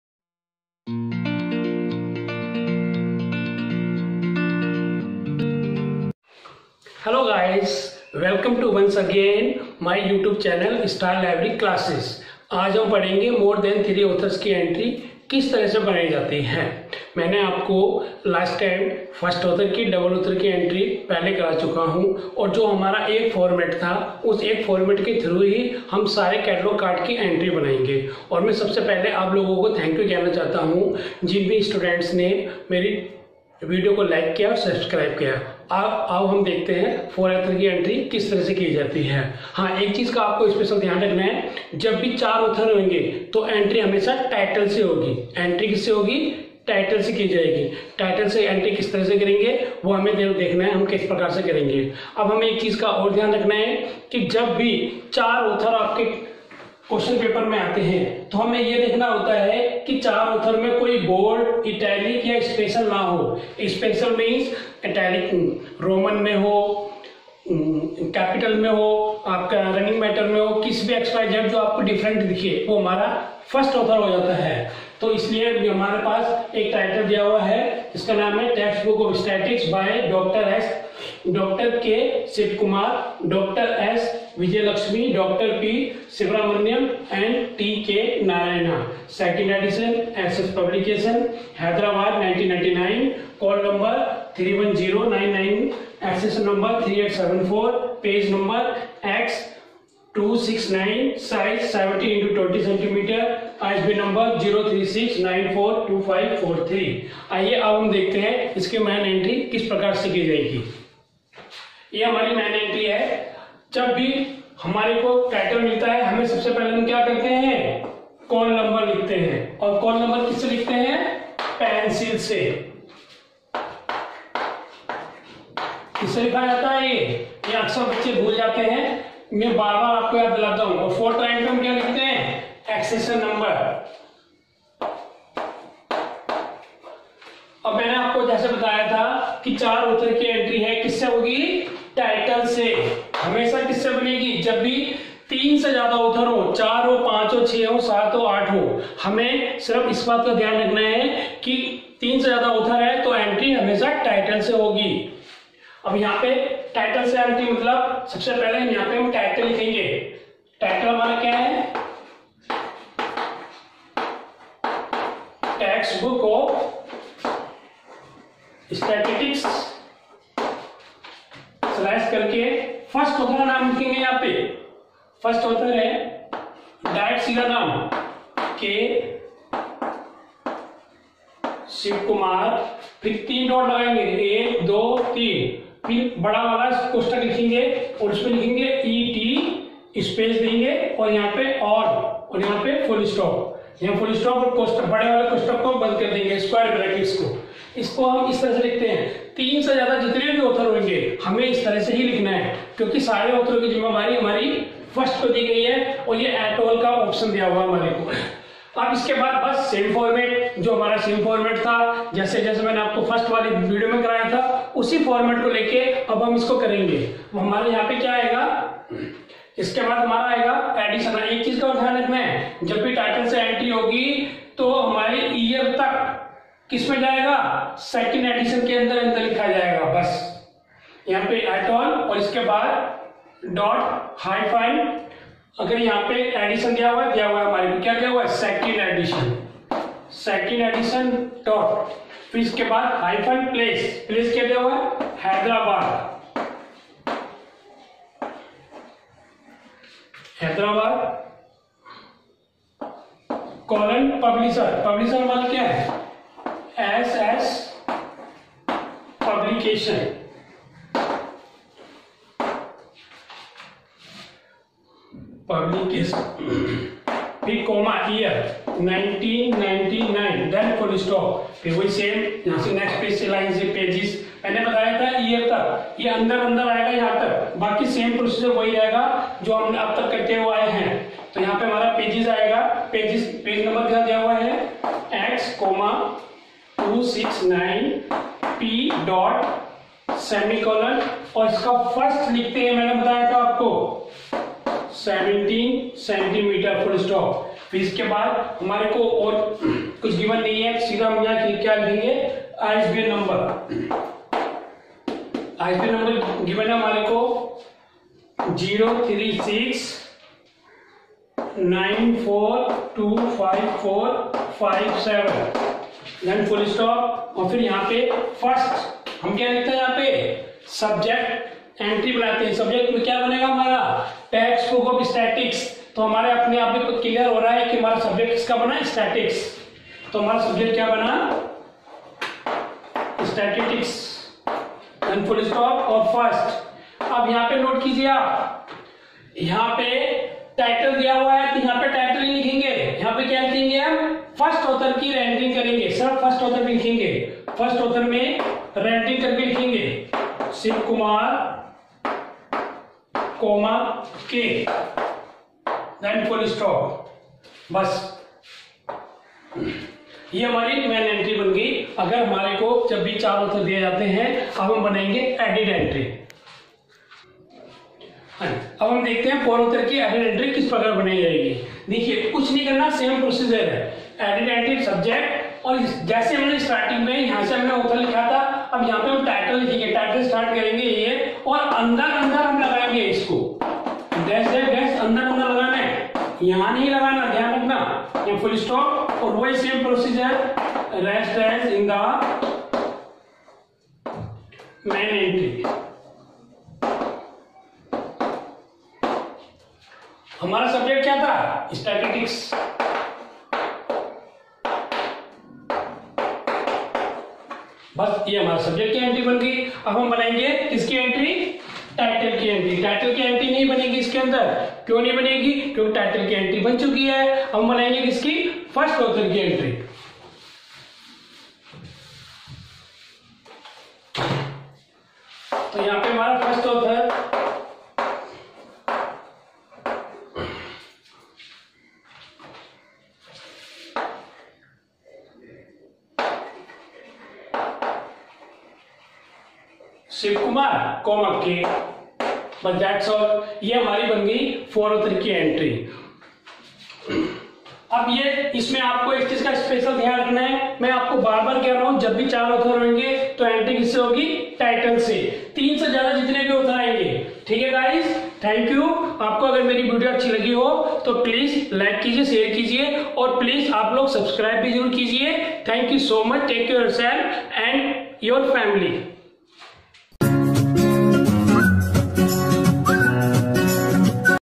Library Catalogue Card (More Than Three Authors) of AACR-II by Subhash Sir
Complete Information of Library Catalogue Card AACR-II
Entry by Author
Format of Library Catalogue Card
Main Entry and Added Entry

Library Catalogue card (Single Author) of AACR-II

Library Catalogue Card (Two Or Three Author) of AACR-II